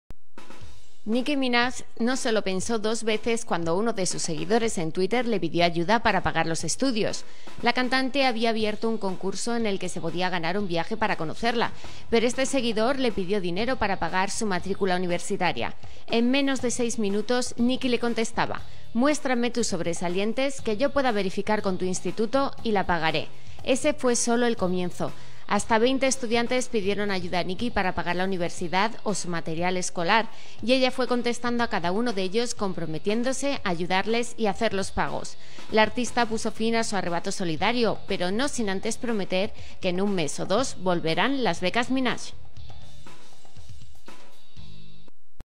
Nicki Minaj pagará los estudios de 20 de sus fans
todo comenzó cuando Nicki inicio un sorteo para regalar un viaje para sus fans y uno de ellos sugirió le ayudara a costear sus estudios, lo que termino con un Gran gesto de la cantante!